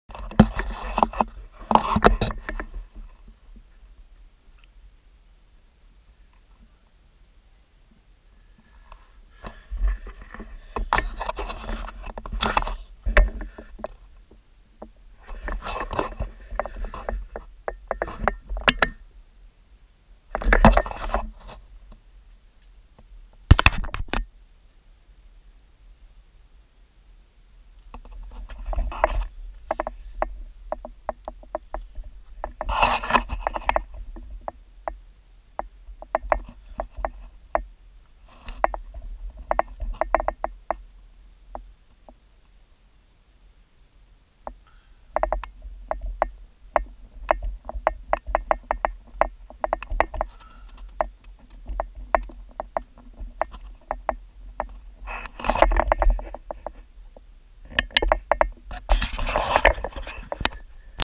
small cave 1, near Oregon Caves, 1520p60
This is one of the small caves near Oregon Caves in southern Oregon.